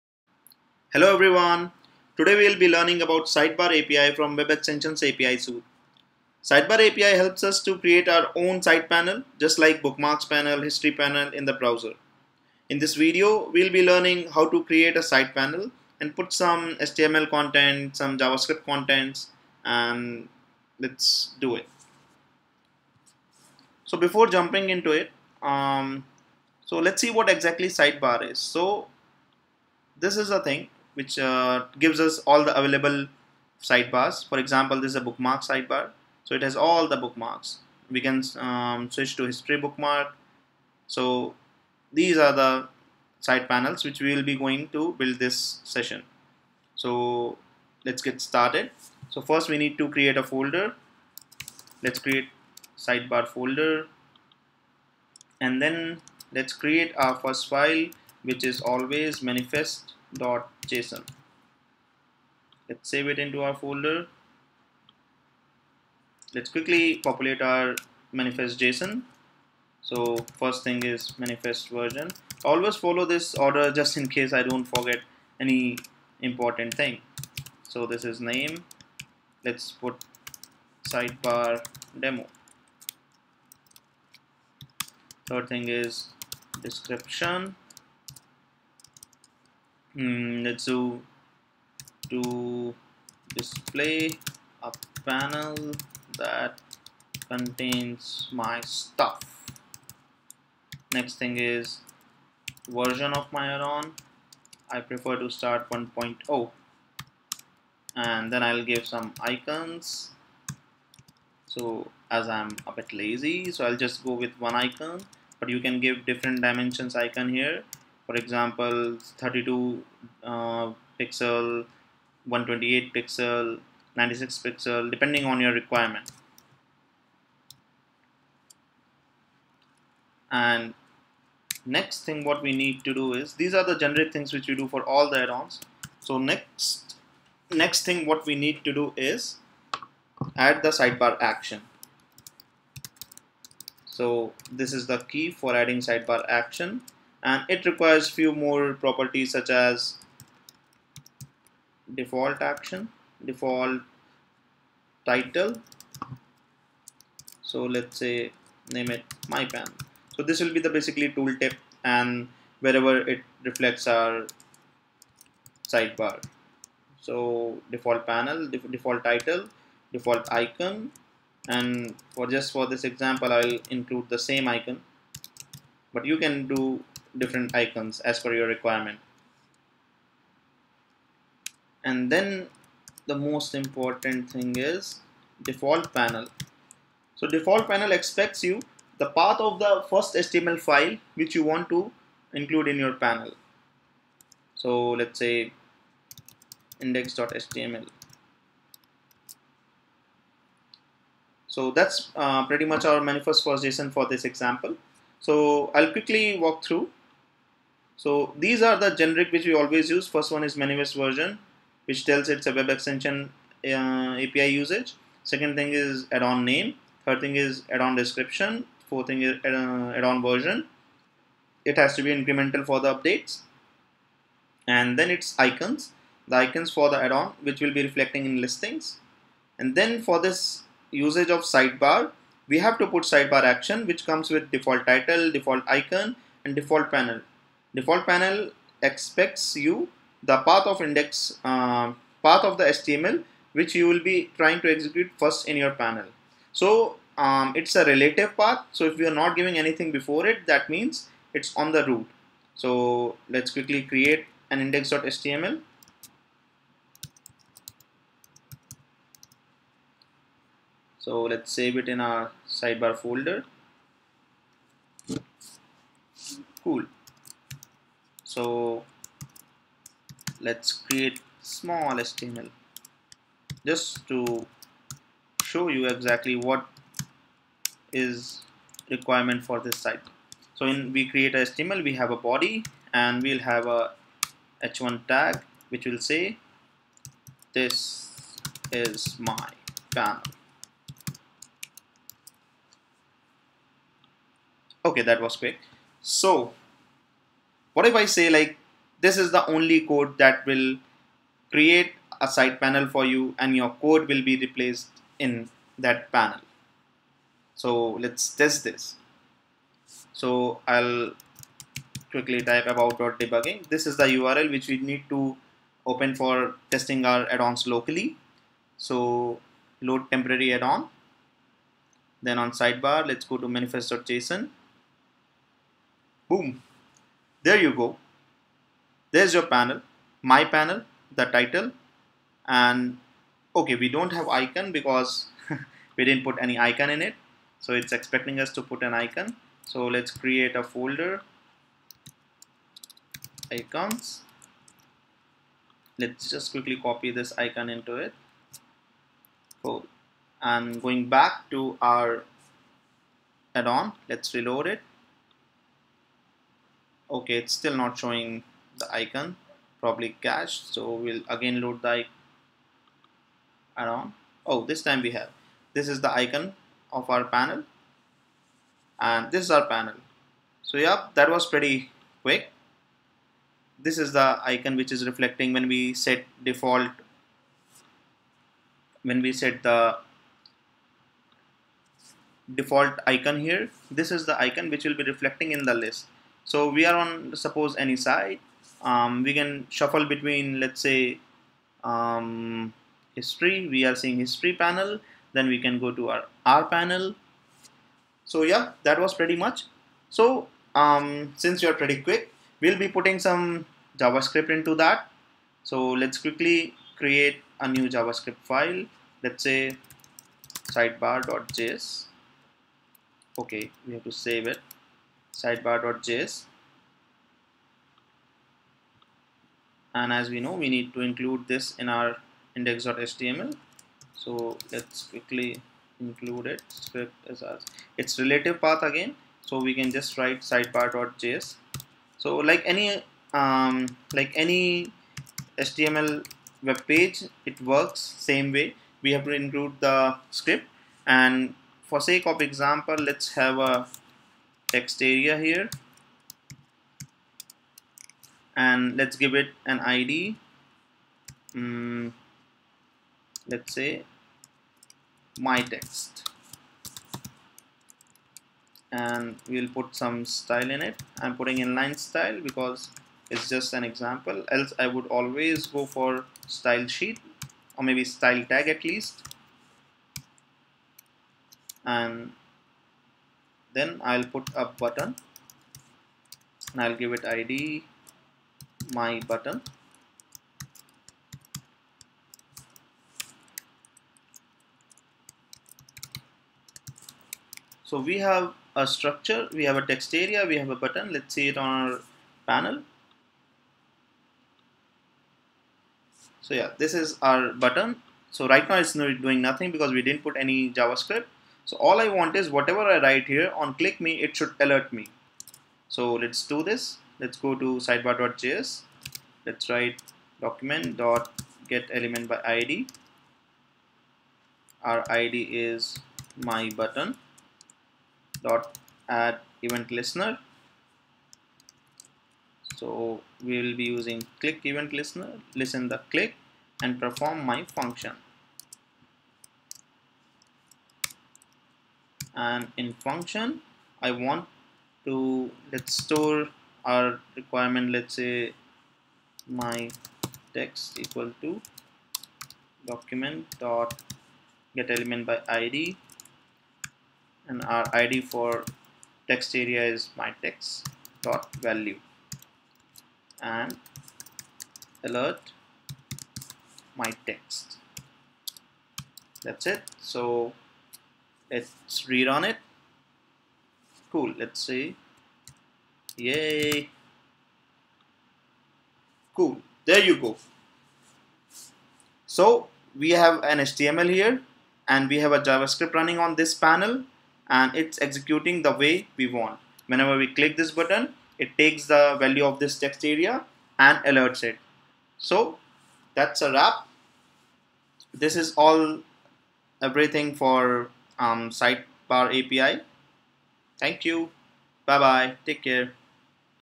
sideBar API - Firefox WebExtensions API
A tutorial to understand sideBar API. This video explains how to add a sidebar to our browser, just like bookmarks panel, history panel, we can add our own sidebar panel and add few HTML and JavaScript code to that panel.